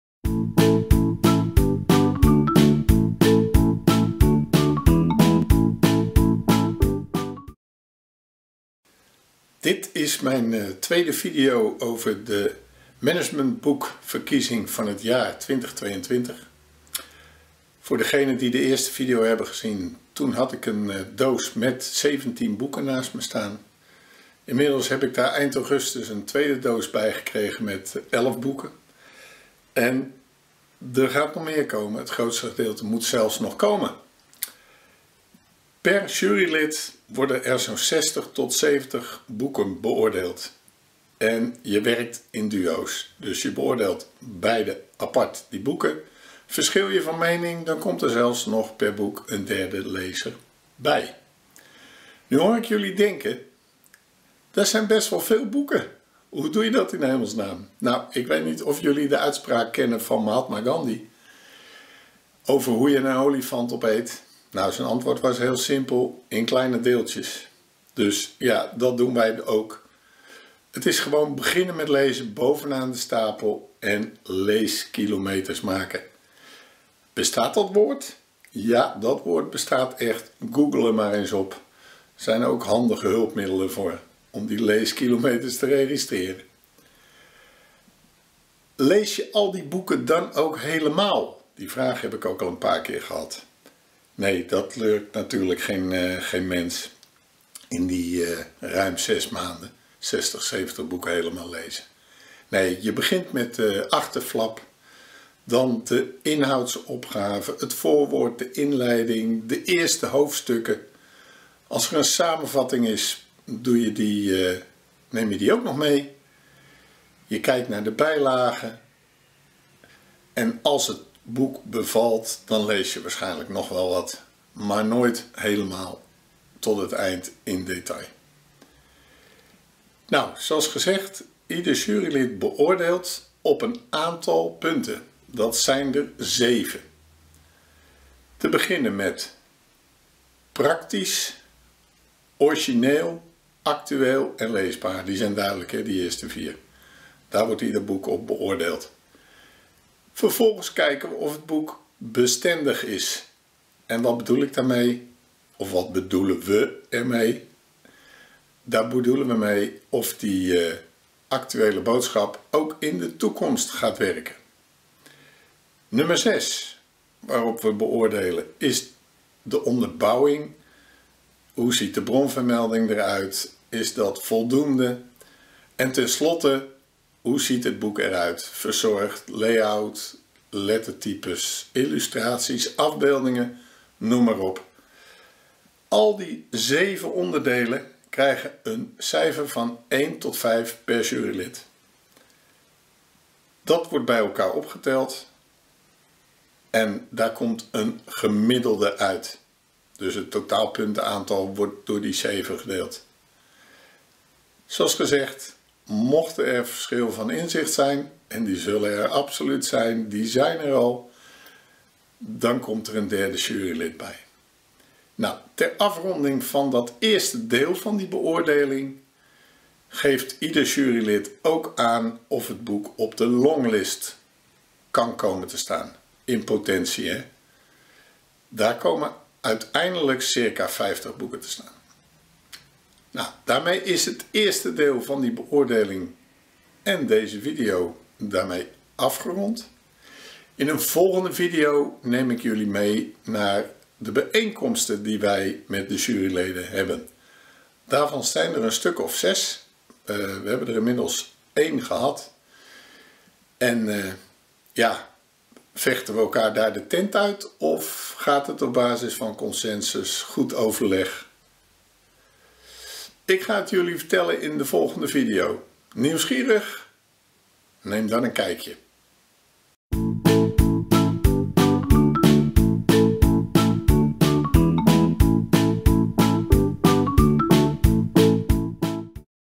Peter Streefkerk over de Managementboek van het Jaar 2022 verkiezing - Deel 2
In deze vlogserie neemt jurylid Peter Streefkerk ons mee in het juryproces rondom de verkiezing van het Managementboek van het Jaar 2022.

In deze video legt Peter uit hoe waar hij allemaal op let als jurylid bij het lezen van de boeken.

Natuurlijk zit Peter Streefkerk niet alleen in de jury. Ook Liesbeth Tetteroo, Louis Thörig, Mirella Visser, Nora Ghaoui en Patrick Davidson nemen daar dit jaar in plaats.